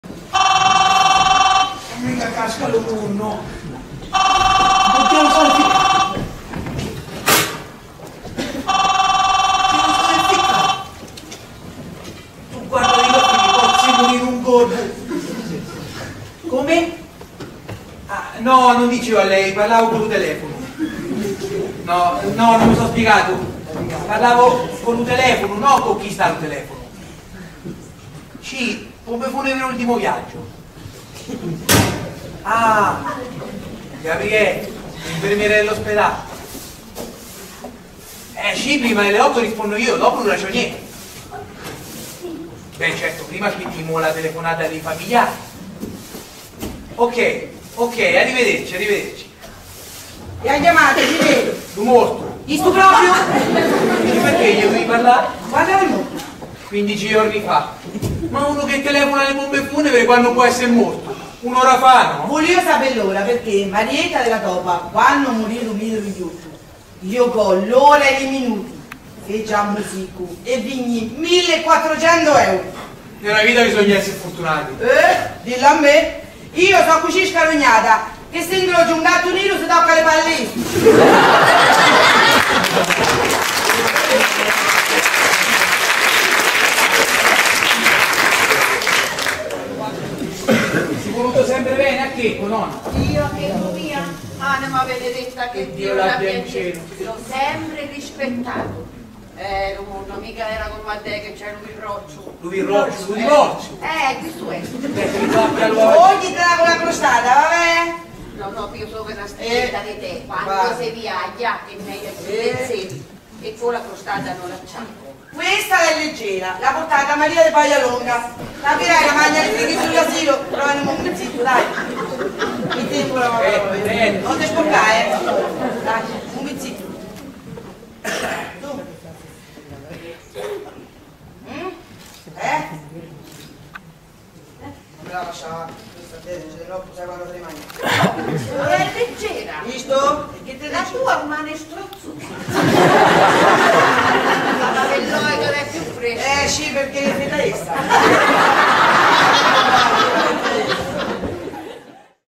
QUALCHE BATTUTA SIMPATICA DALLA COMMEDIA "Le Rughe dell'Anema"
Teatro Velluti di Corridonia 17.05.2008